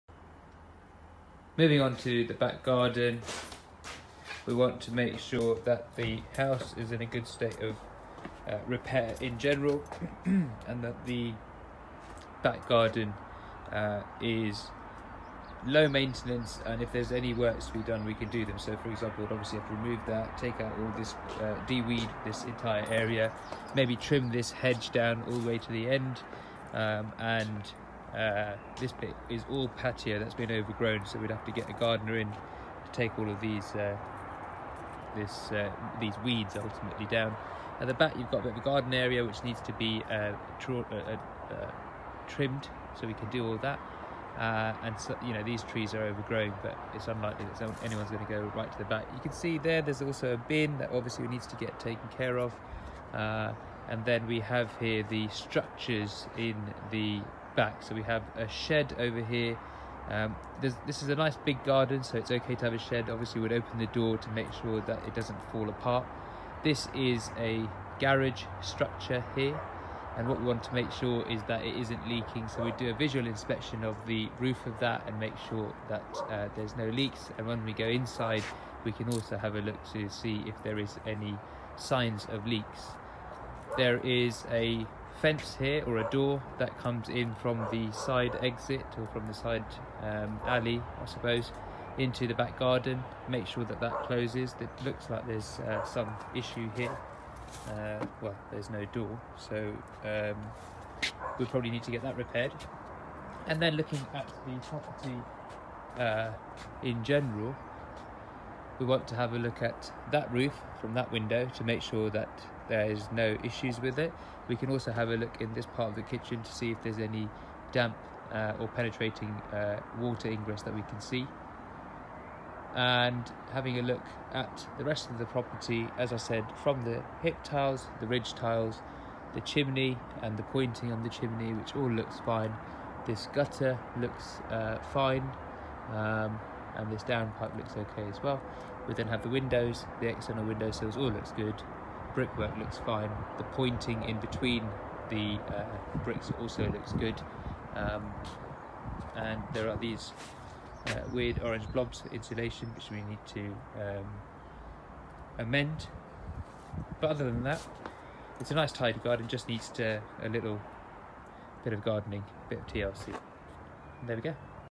How to carry out a buy to let property viewing I Part Three | Property Viewing Checklist
Knowing what to look out for when viewing a BTL property could be the difference between a profitable investment and a money pit!

Have you ever wondered what you need to look out for on the outside of a property when viewing it?

As part of a 3 part series, we take you on a tour of a recent property we viewed in Leeds and show you how we do property viewings  👀

In today's video:

Part 3 : What to look for in the gardens and at the back of the property
Patios & gardens
Hedges
Fences & access to the garden
Outbuildings
Roof
Guttering
Windows
External Doors

If you’d like to know more about what we do, register your interest today to receive monthly property news, investment insights, and market updates.